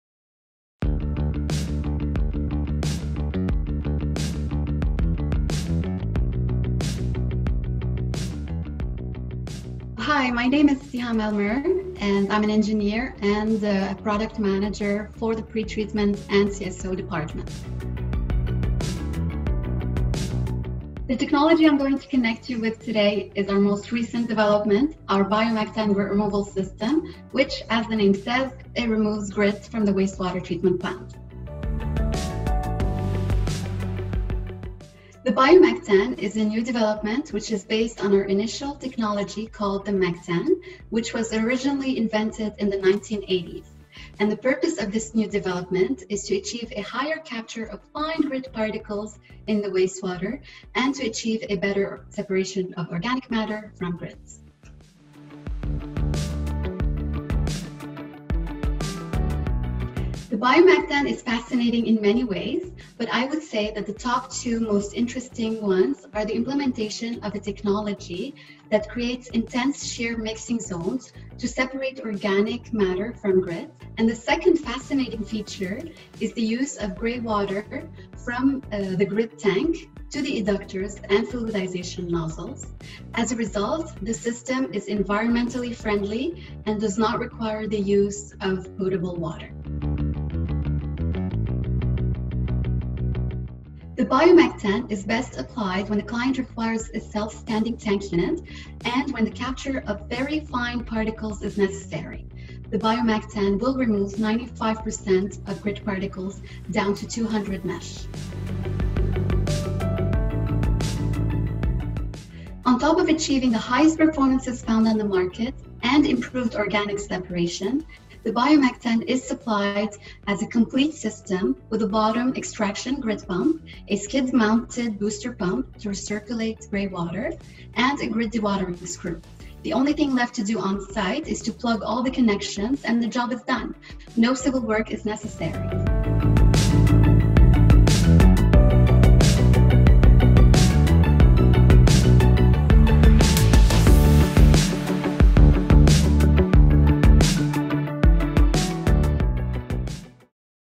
Connect with BioMECTAN™
In this Veolia Water Tech Talk, connect with technology expert, Siham El Murr, as she shares how the BioMECTAN® is taking grit removal to the next level.  This advanced hybrid vortex grit removal system is achieving superior grit removal rates to better protect downstream equipment and treatment processes. Watch this video to learn how the BioMECTAN® can help improve your headworks process.